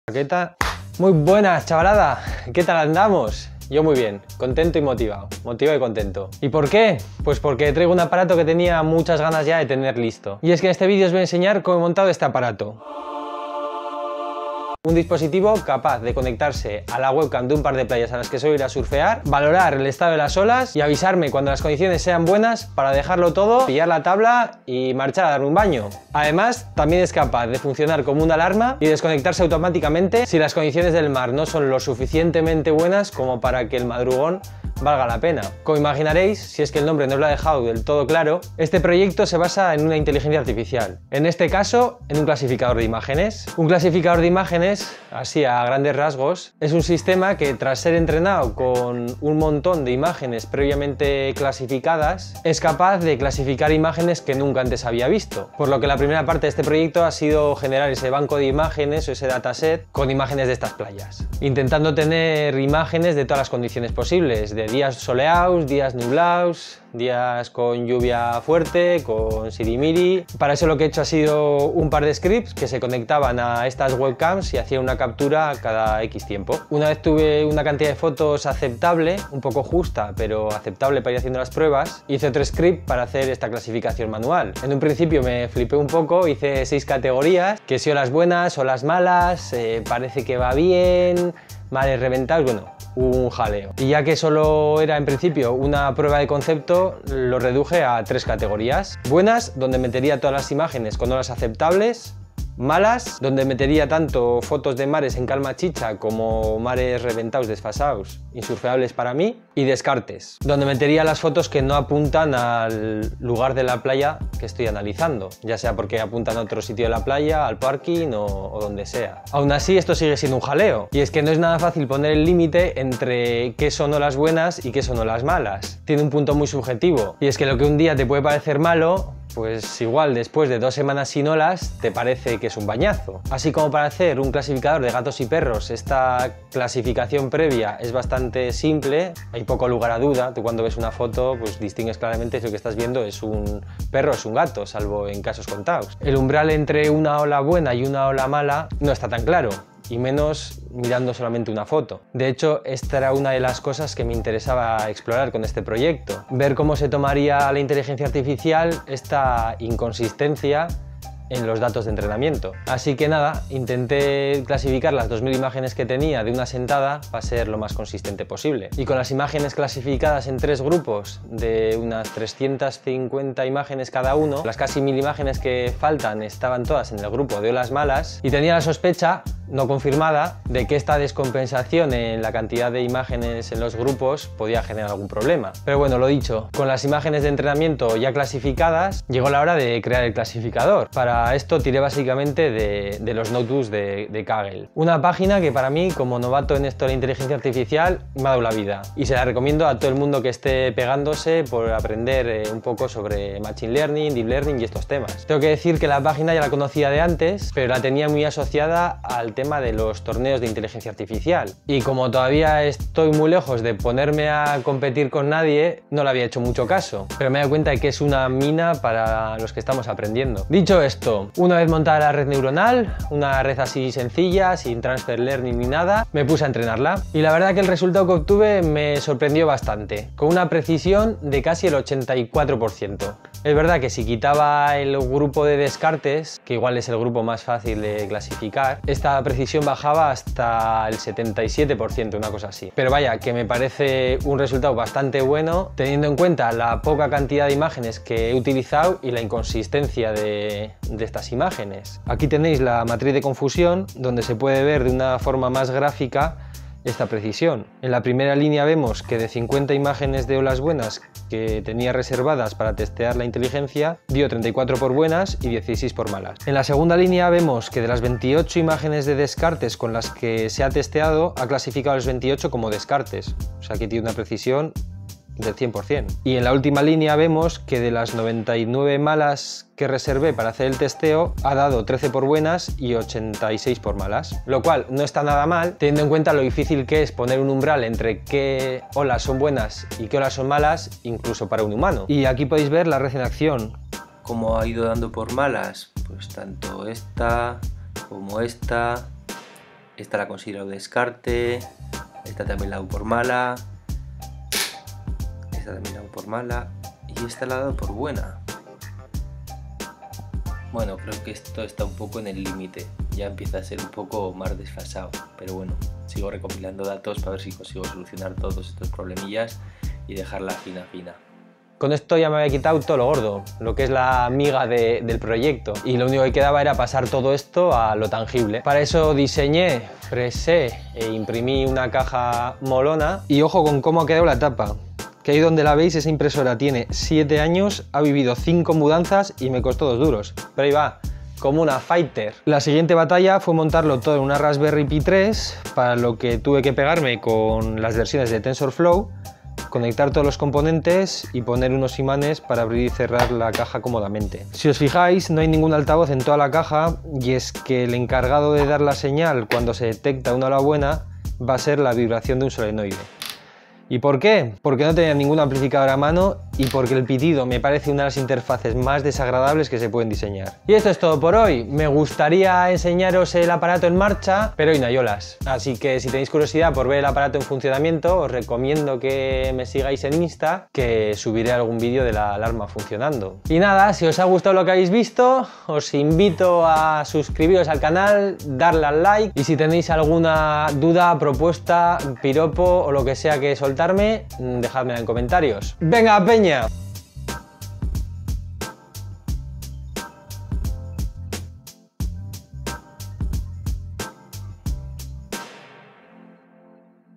bAIwatch 🌊 Inteligencia Artificial Surf y Raspberry Pi
bAIwatch es un dispositivo capaz de conectarse a la webcam de una playa cercana a mi casa, analizar las imágenes y avisarme en caso de que las condiciones sean buenas como para ir a surfear.

Os dejo también el link la sesionaza de Panda Dub de la que he extraído el loop que suena de fondo: youtu.be/u5Vs4Yw9HTM